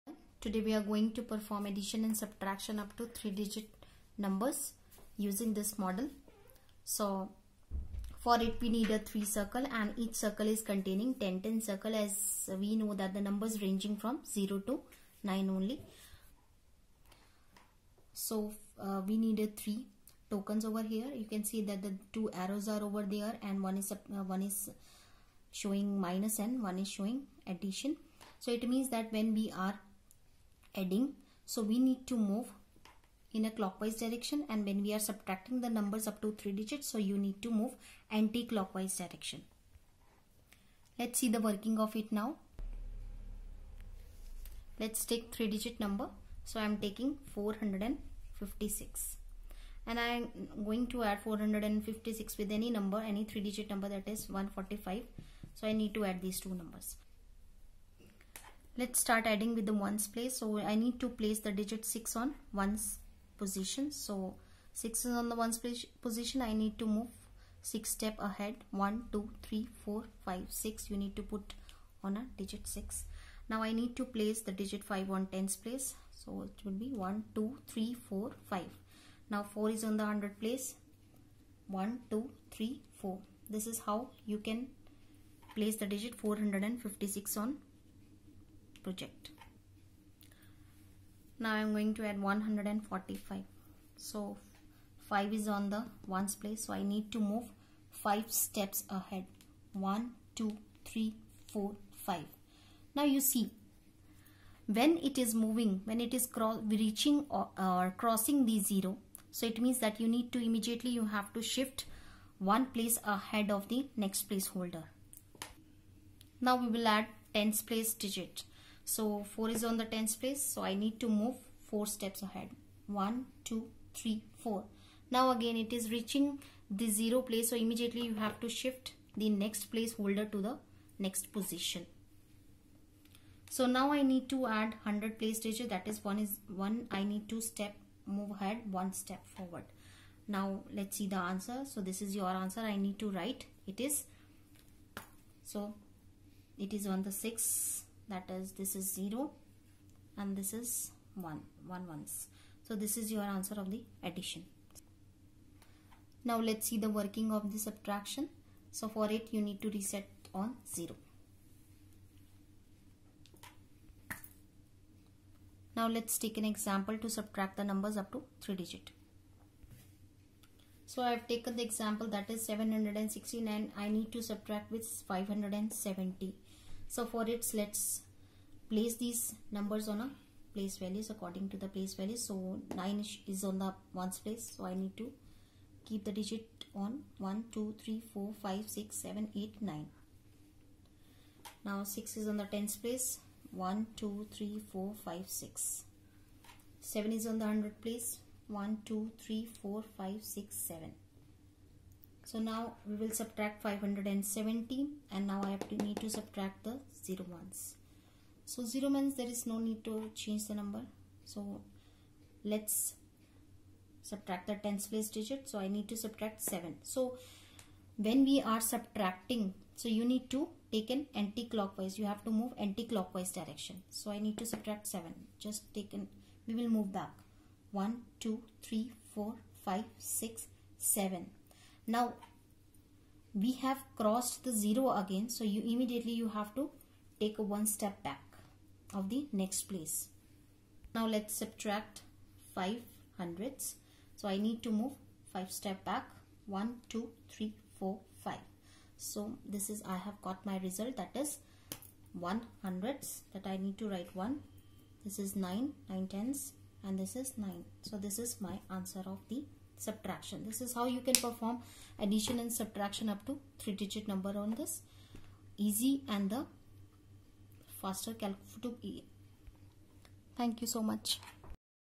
Addition and subtraction calculator
This video help u alot in adding and subtracting the numbers .
Easy and simple working of this calculator will make u learn maths calculations in a fun way!!

Let's see the working.

ADDITION OF ANY 3-DIGIT NUMBERS( 0-999)

Reset all three tokens on ‘0’ position.
Place each digit of the first number 456 on each circle according to their place values .
The digit 4 should be on Hundreds,the digit 5 on tens,the digit 6 on ones place.
Now, we will add 145 moving  from ones
to hundreds place.
In ones place, we will move the token which was placed on the digit 6 5 steps ahead to add (6 + 5), we will see the token will reach and cross the zero ( 0) position so, we will immediatedly shift the token of tens place digit by 1.
Now we will add ( 5 + 4) of tens place, we will move 4 steps ahead of the token which  was placed on digit 6.now, the token will reach on zero ( 0) position, we will shift the hundreds place token by 1 digit.
Similarly, we will add ( 4 + 1) of hundreds place, we will move 1 step ahead of the token which was placed on digit 5.
The position shown by the tokens after performing the above steps is the sum of the numbers 456 + 145.

SUBTRACTION OF ANY 3 –DIGIT NUMBERS ( 0 -999)

Place each digit of the first number 769 on each circle according to their place values .
The digit 7 should be on Hundreds,the digit 6 on tens,the digit 9 on ones place.
Now, we will subtract 769 moving  from ones
                to hundreds place.
In ones place, we will not change the position of the token to subtract ‘0’.The digit 9 will be on the same place.
Now we will subtract ( 6 -7 ) of tens place, we will move 7 steps back  in anti-clockwise direction of the token which  was placed on digit 6.
We will observe that when we are moving anti- clockwise ,the token will cross the  zero ( 0) position, and will reach on the digit 9 .
When it happens, so we will immediately shift the hundreds position of the token 1 step back.
 Similarly, Now we will subtract  ( 7-5) of hundreds place, we will move 5 step back of the token which was placed on digit 6.
The position shown by the tokens after performing the above steps is the difference of the numbers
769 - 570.